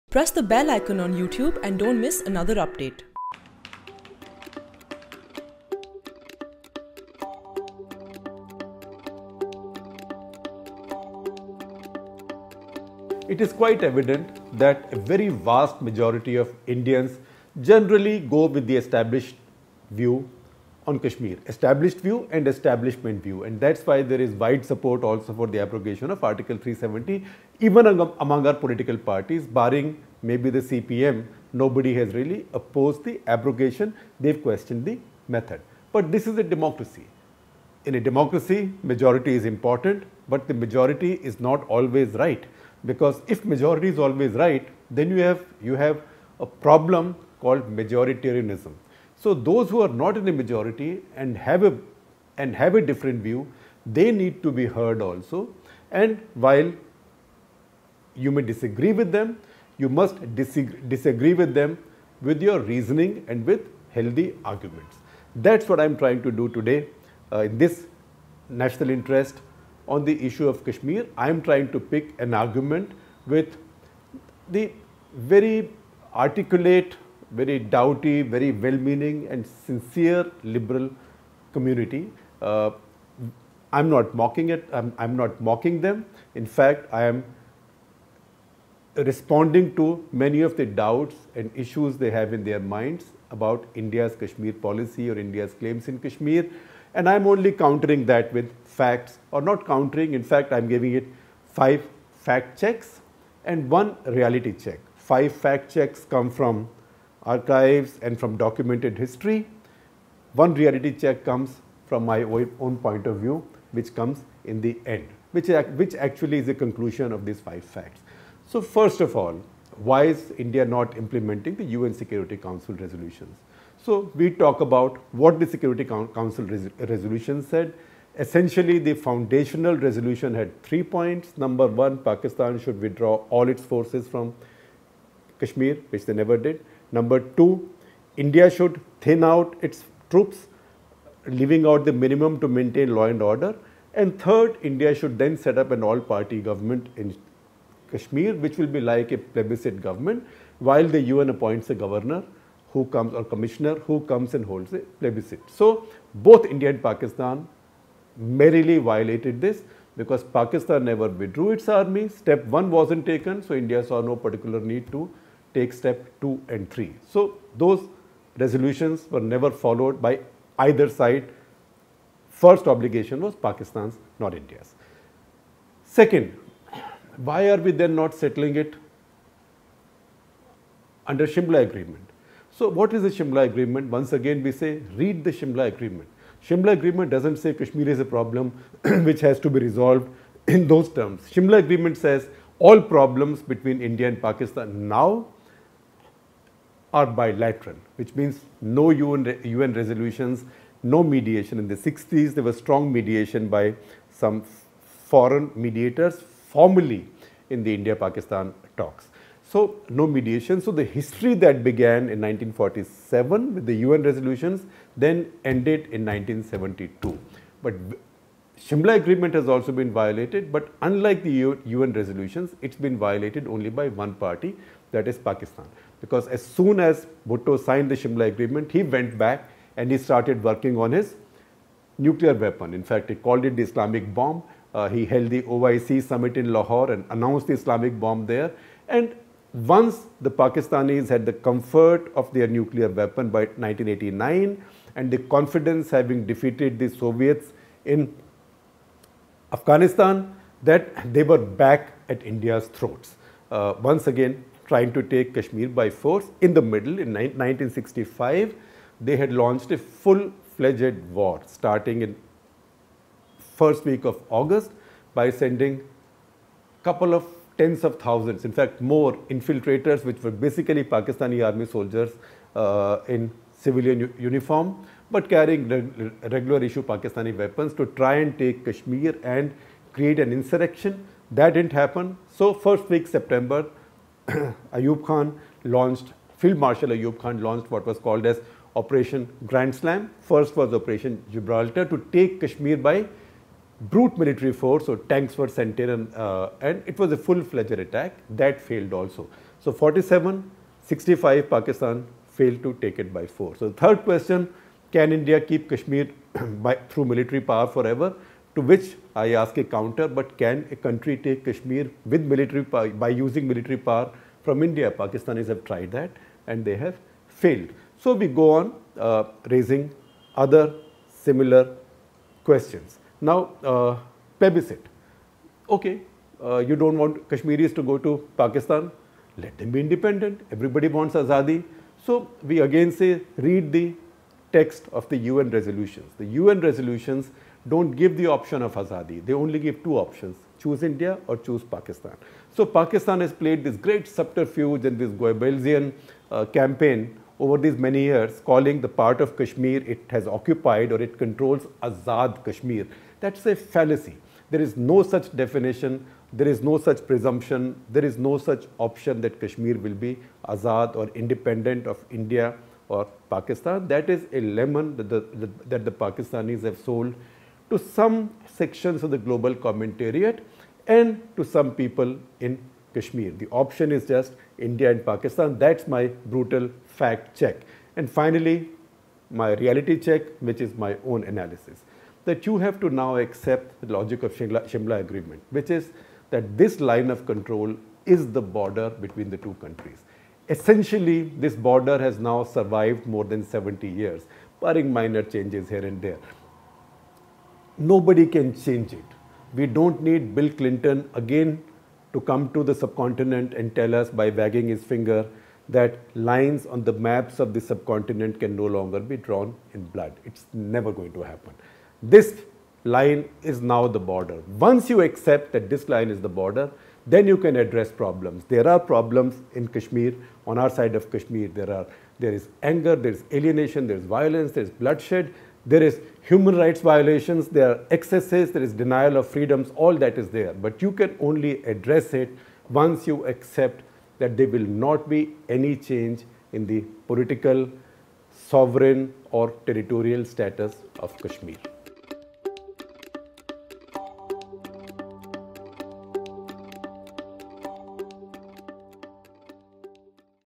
To understand Modi’s new Kashmir reality, these 5 liberal myths need to be broken
Kashmir has a new status quo & Pakistan can risk breaking it. A solution can be found only after accepting that the existing borders are permanent.